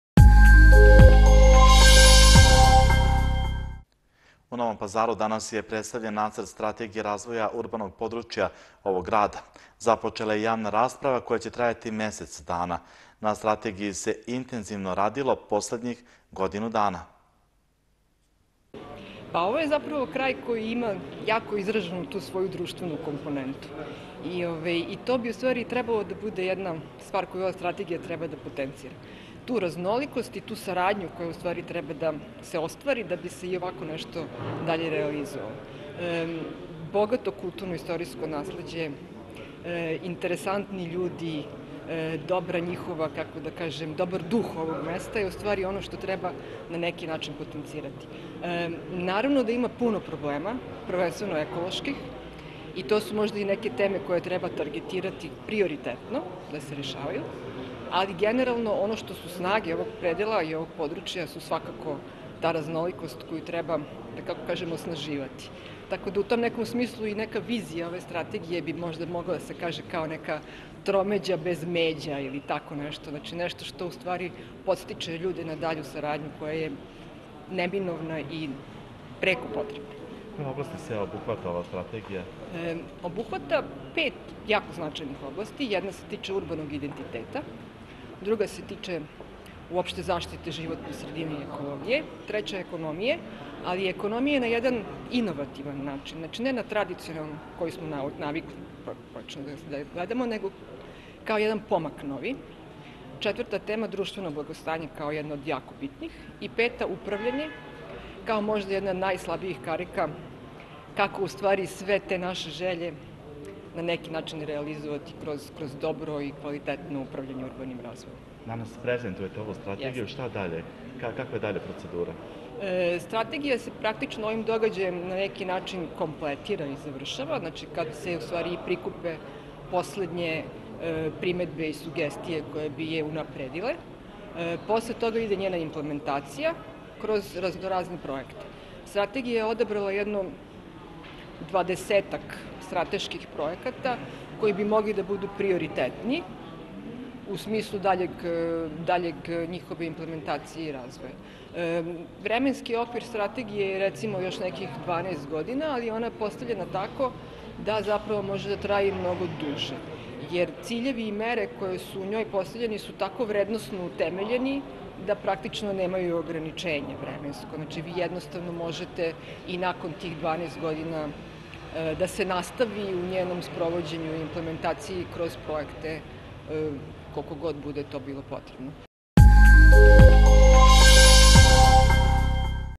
Prezentovan nacrt Strategije razvoja urbanog područja Novog Pazara
U Novom Pazaru danas je predstavljen nacrt Strategije razavoja urbanog područja ovog grada. Započela je i javna rasprava, koja će trajati mesec dana. Na strategiji se intenzivno radilo poslednjih godinu dana.